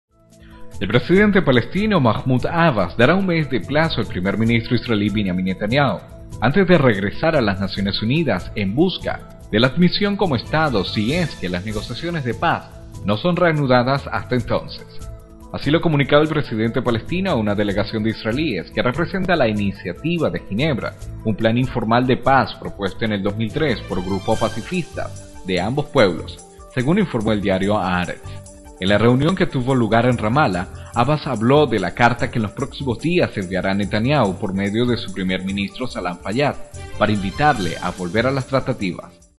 Abbas dará un mes de plazo a Netanyahu
El presidente palestino, Mahmud Abbás, dará un mes de plazo al primer ministro israelí, Benjamín Netanyahu, antes de regresar a las Naciones Unidas en busca de la admisión como Estado si es que las negociaciones de paz no son reanudadas hasta entonces.
Así se lo comunicó el presidente palestino a una delegación de israelíes que representan a la Iniciativa de Ginebra, un plan informal de paz propuesto en 2003 por grupos pacifistas de ambos pueblos, según informó el diario «Haaretz».

En la reunión, que tuvo lugar en Ramallah, Abbás habló de la carta que en los próximos días enviará a Netanyahu por medio de su primer ministro Salam Fayyad, para invitarle a volver a las tratativas.